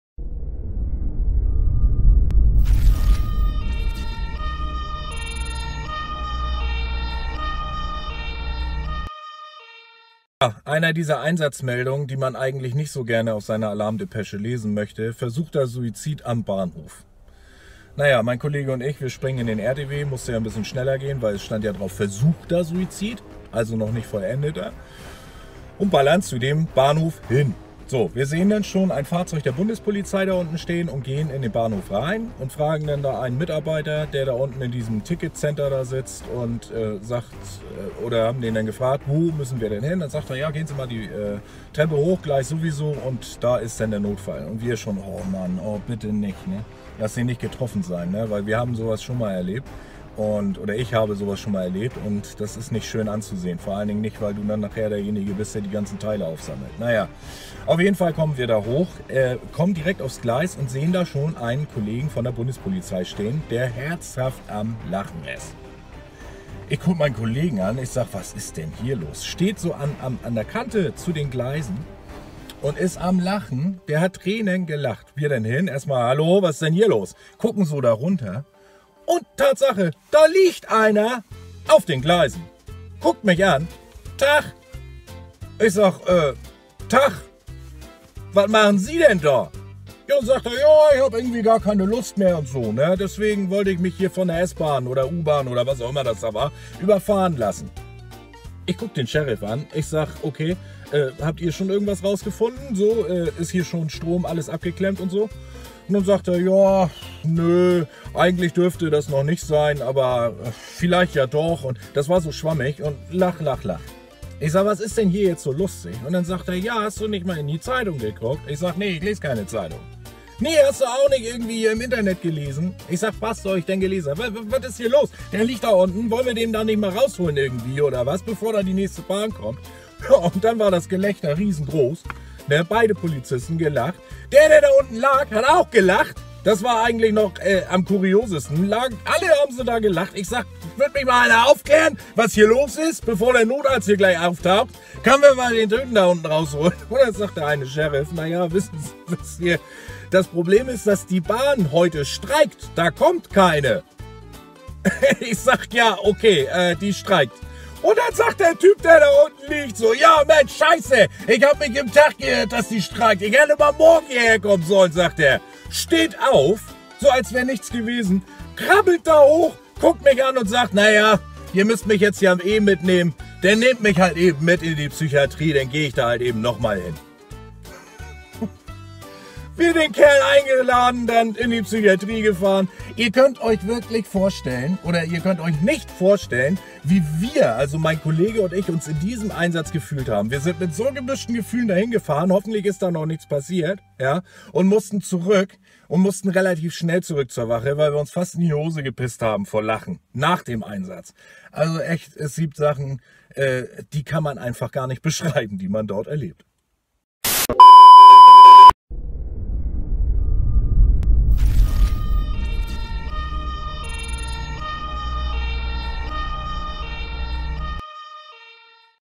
Der Mann auf den Bahngleisen
Ein Horroreinsatz für jeden Retter:
"Versuchter Suizid am Bahnhof - Person auf Bahngleis"
Dieser Einsatz sollte meinem Kollegen und mir noch lange im Gedächtnis bleiben...
Das Sievi-FON wartet auf eure Anrufe: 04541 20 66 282 (zum Normaltarif!)